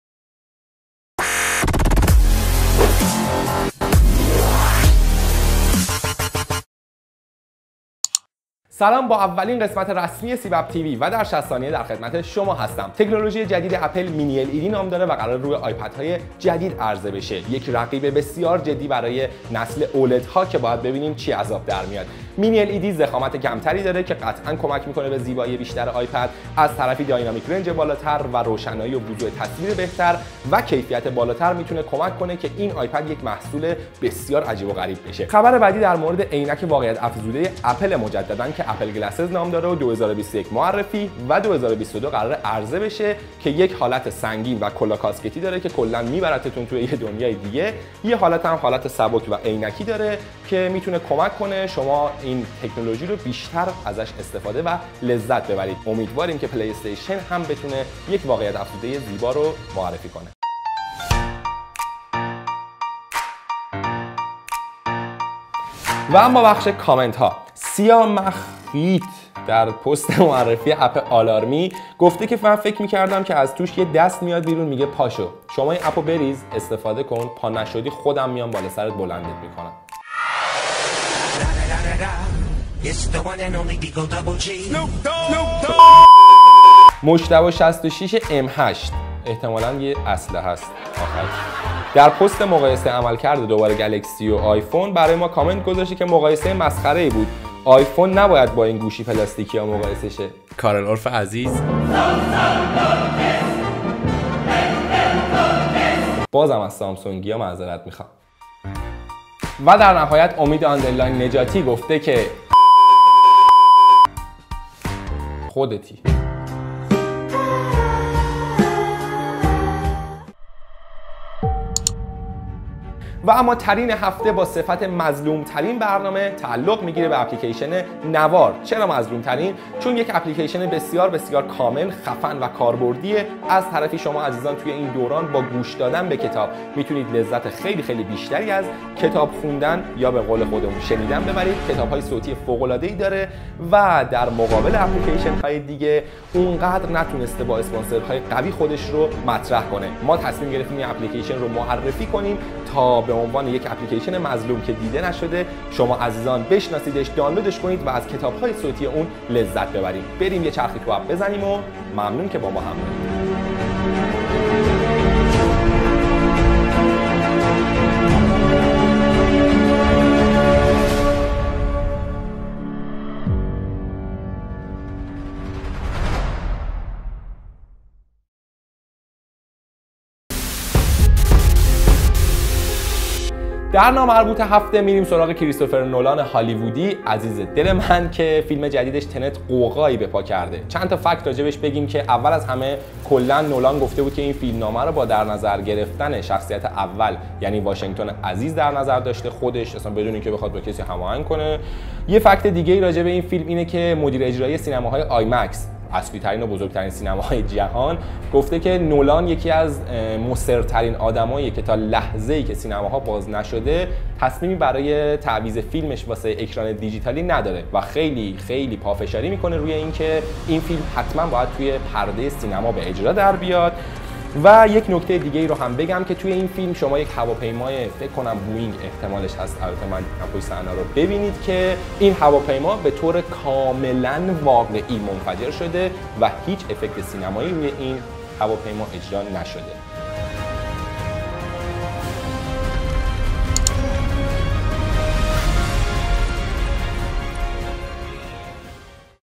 به دومین برنامه فصل دوم سیب‌اپ تی‌وی خوش آمدید
علاوه بر بخش‌های جذاب، در این قسمت جزئیات بیشتری از عینک واقعیت افزوده اپل یعنی اپل گلسز خواهید شنید.